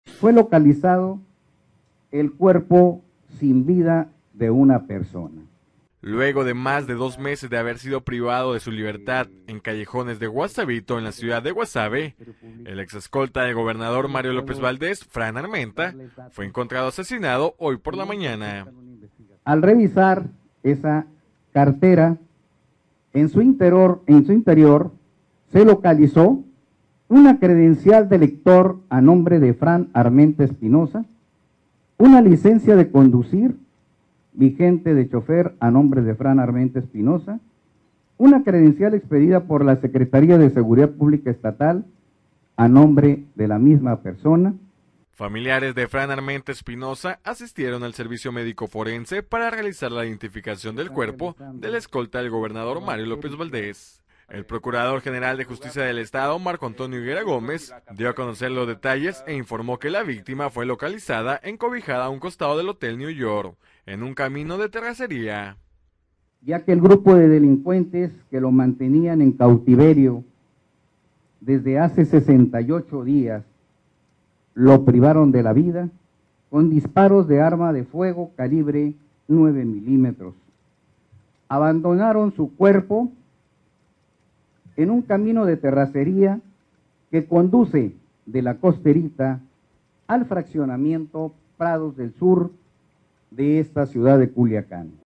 Aparece muerto escolta de Gobernador "Malova" ; había denunciado nexos del gobernador con el narco
El cadáver de Frank Armenta Espinoza, escolta del gobernador de Sinaloa, Mario López Valdez --a quien acusó de proteger al Cártel de Sinaloa en un video--, fue localizado esta mañana en Culiacán, a espaldas del conocido motel Nueva York.